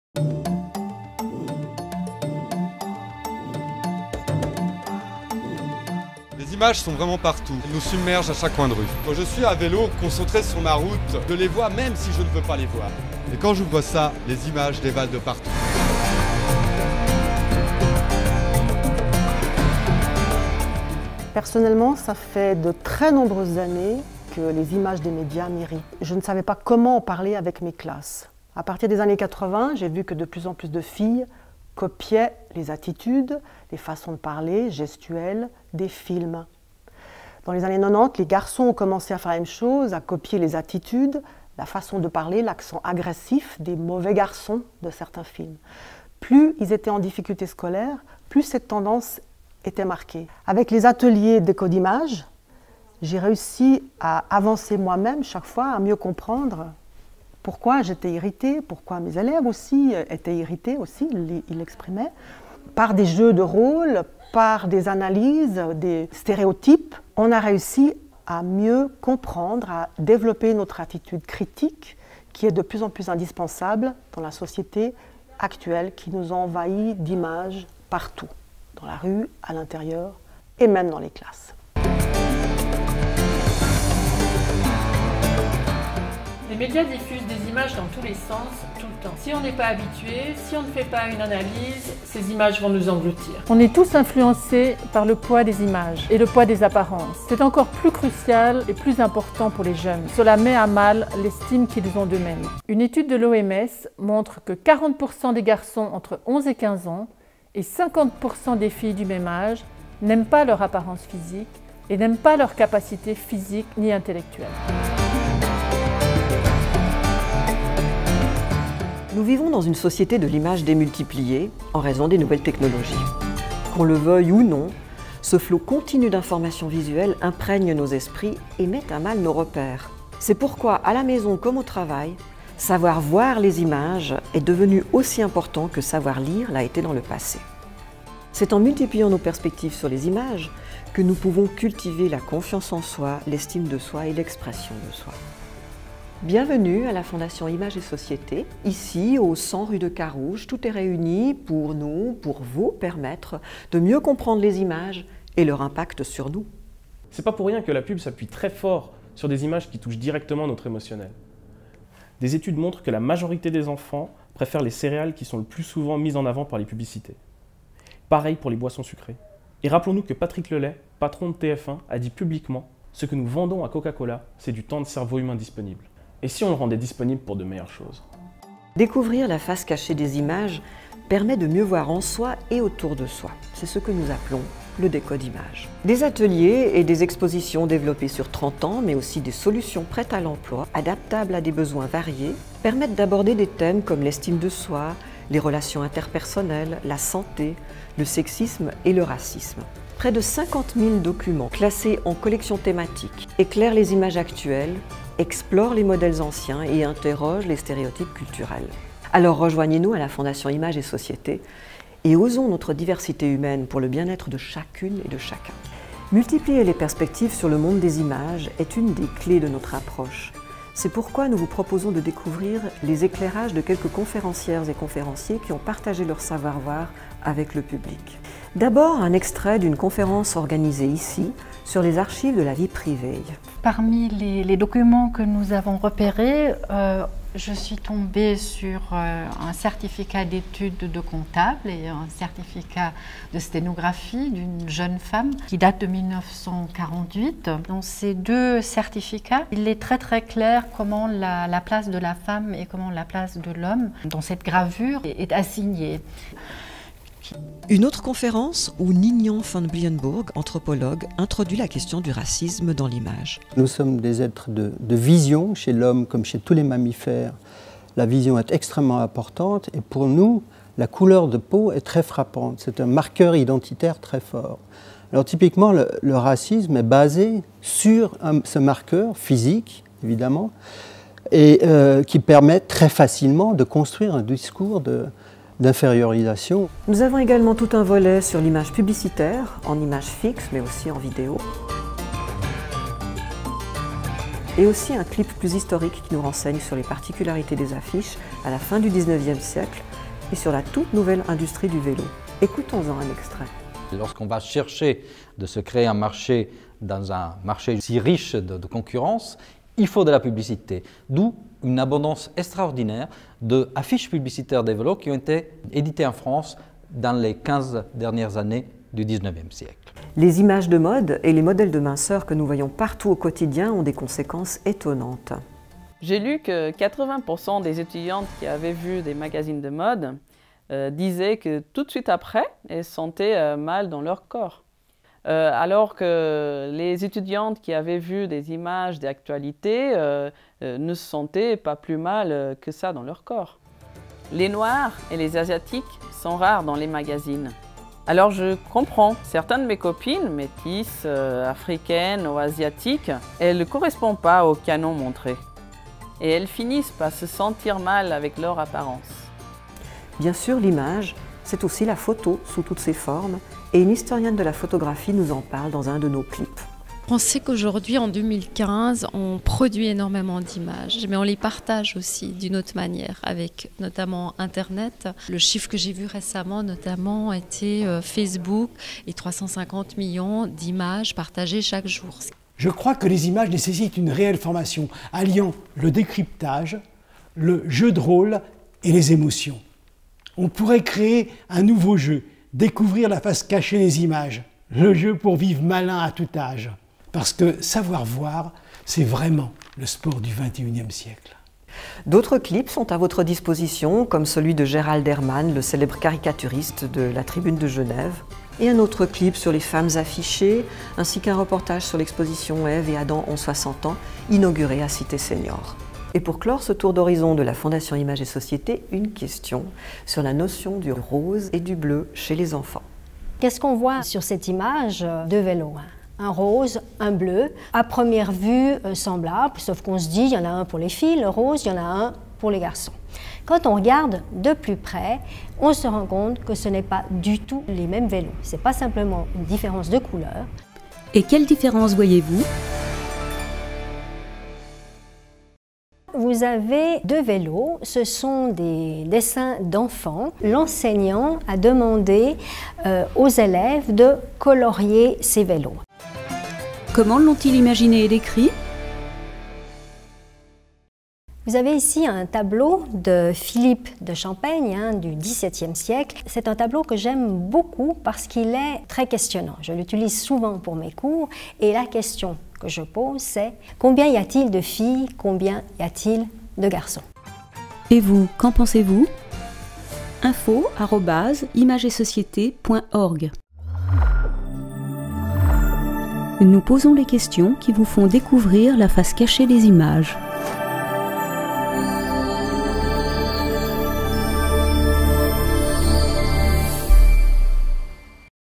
Des activités pour mieux voir et comprendre
Nous sommes tous sous l'influence des images. Pas seulement les enfants.  Pour ne plus être des analphabètes du monde visuel, la fondation images et société vous invite à des activités de "décod'image".  Site internet